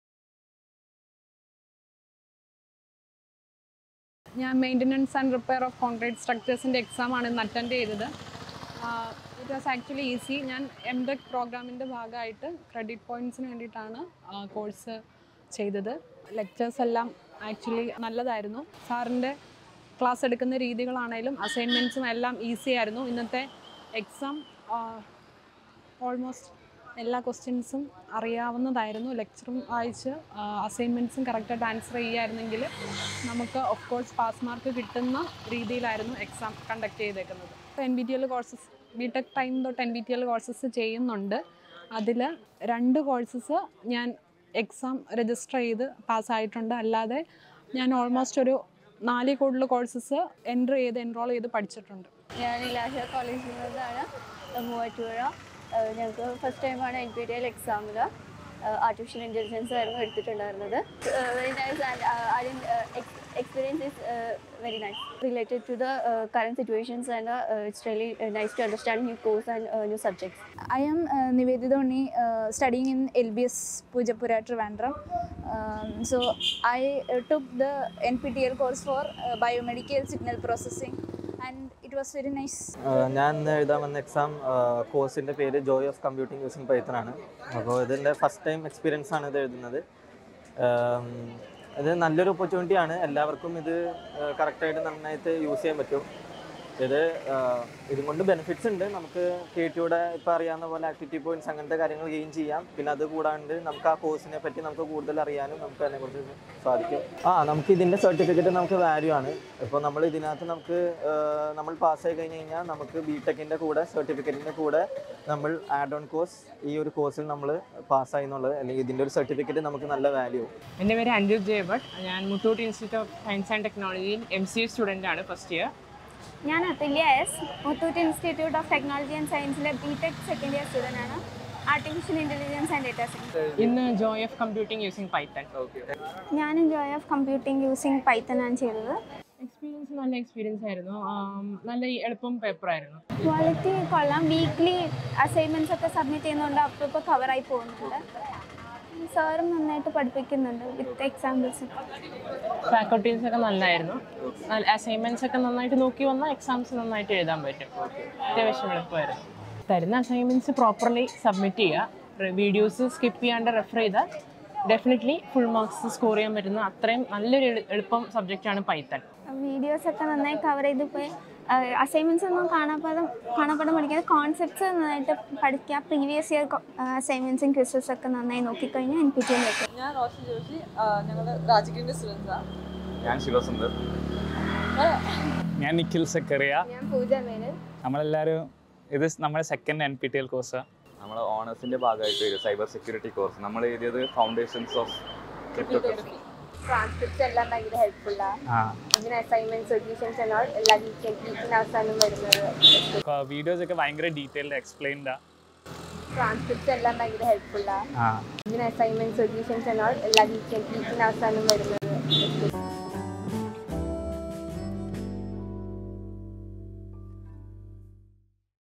Students in Kerala Talk About Their NPTEL Experience '24
Ambitious college students from Kerala share their NPTEL experience as they navigate the challenges and opportunities. Witness their determination and thirst for knowledge as they embrace these online courses, transcending traditional boundaries and unlocking new pathways to academic success.